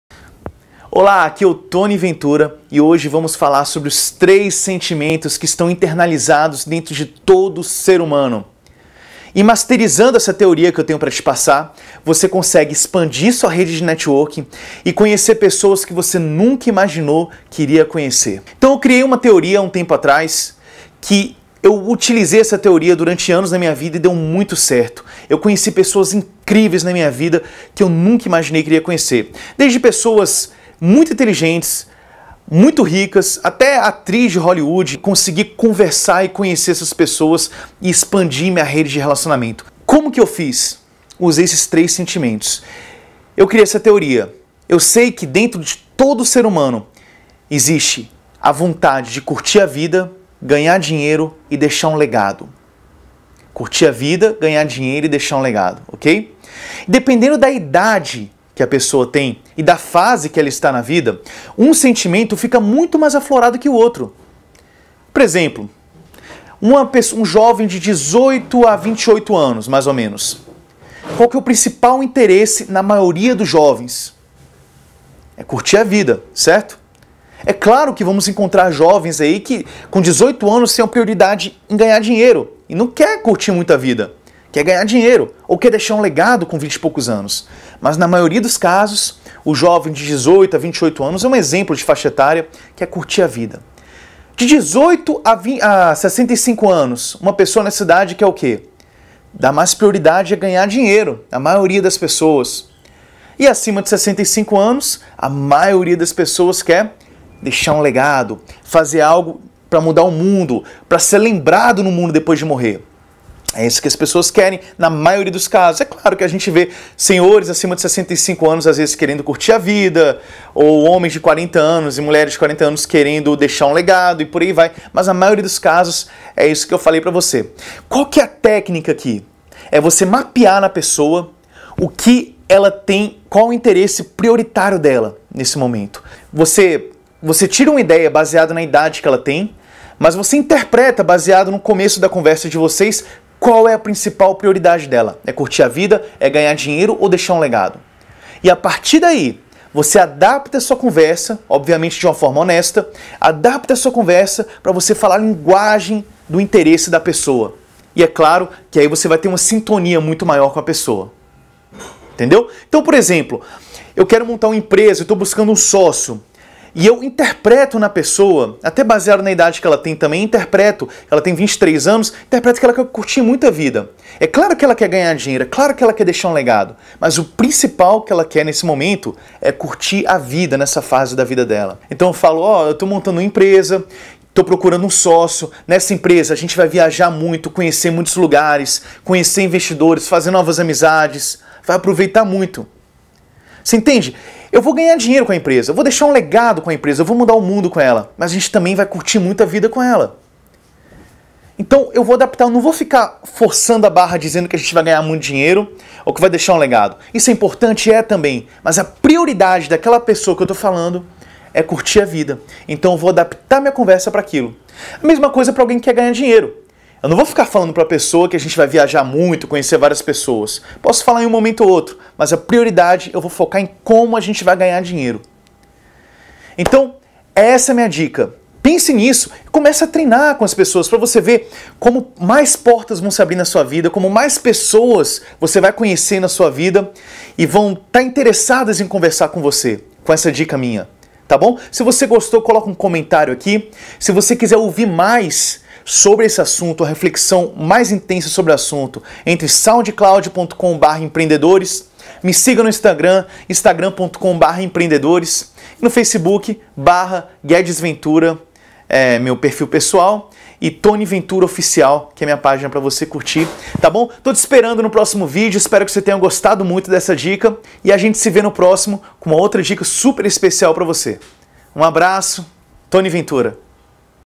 Como ter afinidade instantânea e aumentar sua rede de relacionamento
Todo o ser humano tem 3 sentimento internalizados, curtir a vida, ganhar dinheiro e deixar um legado. Em qual você está?

Novas Tecnologias, Empresas, Startups, empreendedorismo, criatividade - tony ventura